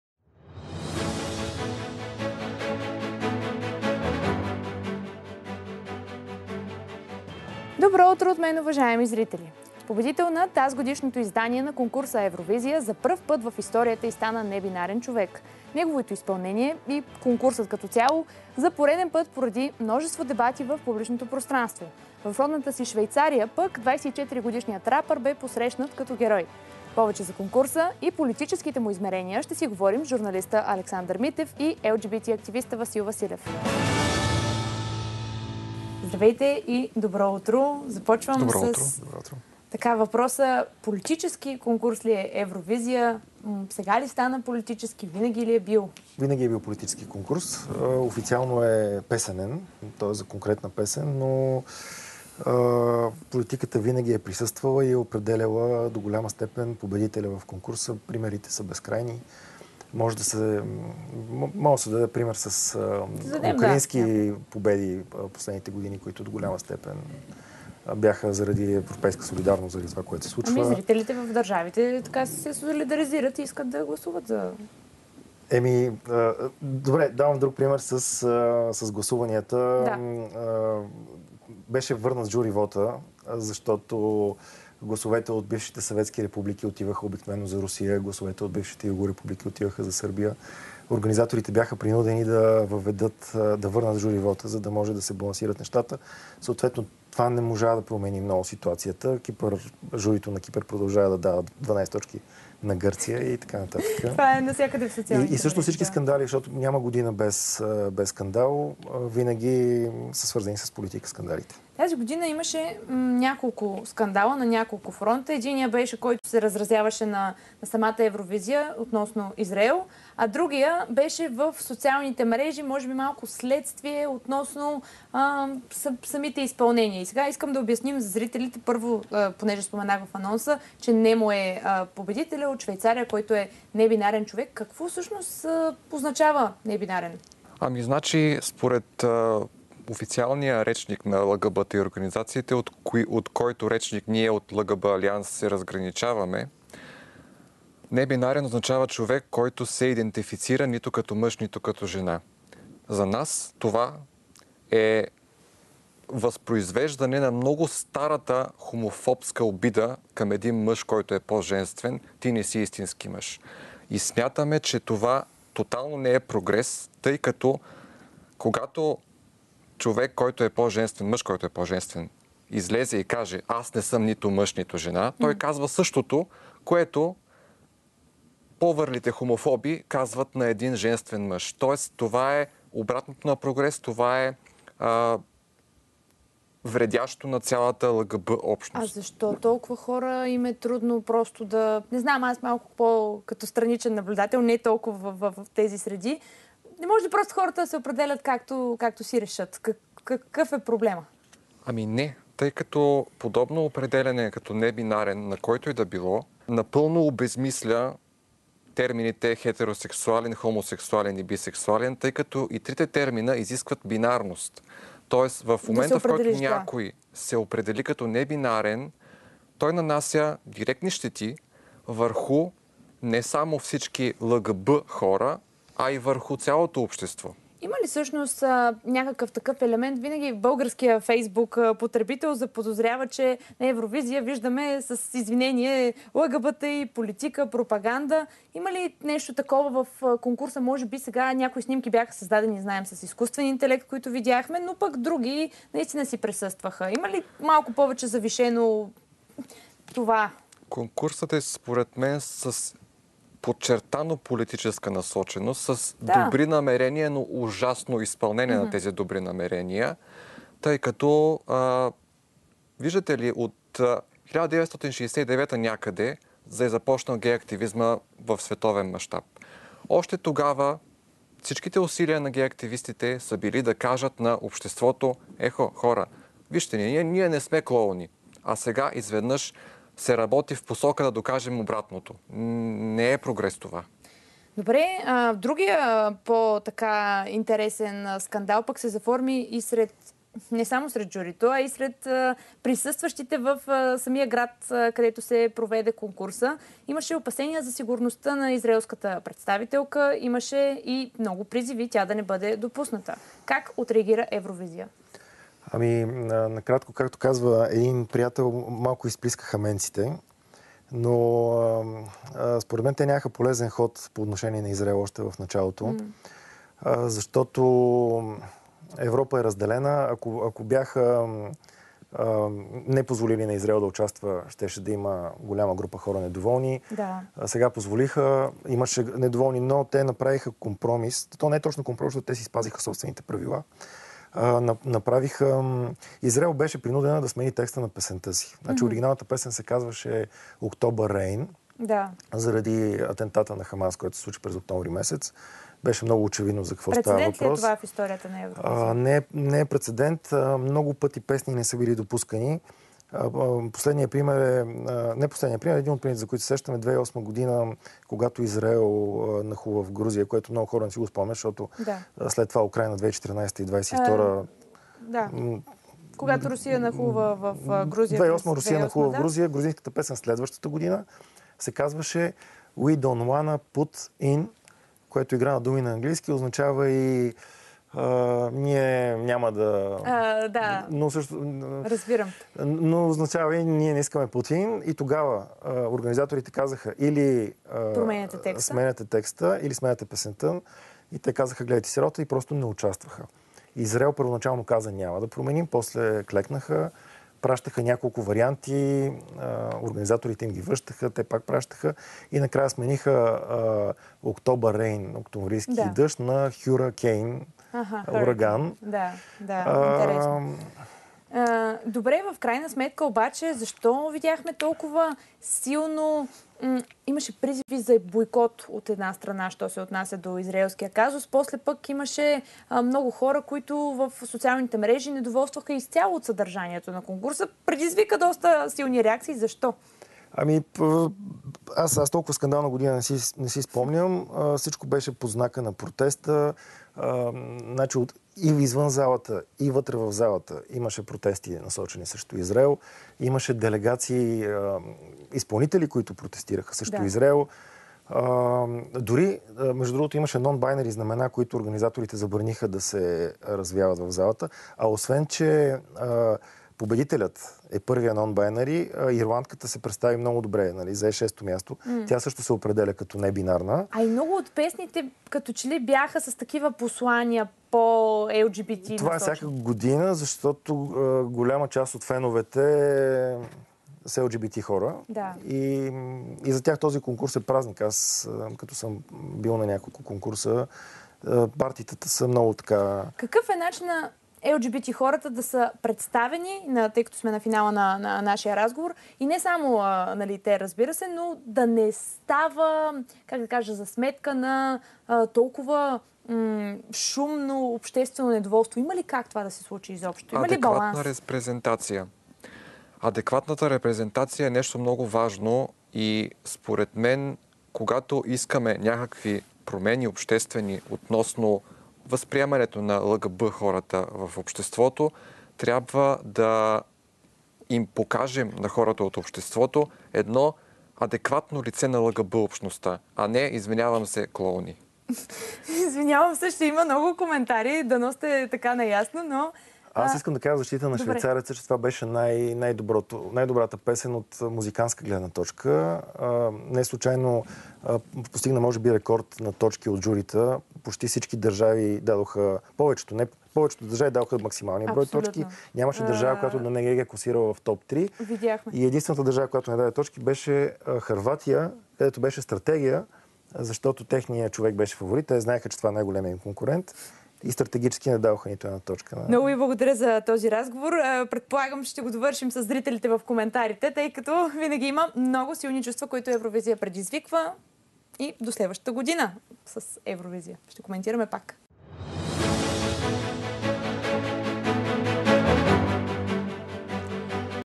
Първият небинарен победител: Немо спечели „Евровизия“
Швейцарецът Немо спечели "Евровизия". Рапърът е първият изпълнител в конкурса, който се идентифицира като небинарен.

Повече за конкурса и политическите му измерения ще научим от Александър Митев и Васил Василев.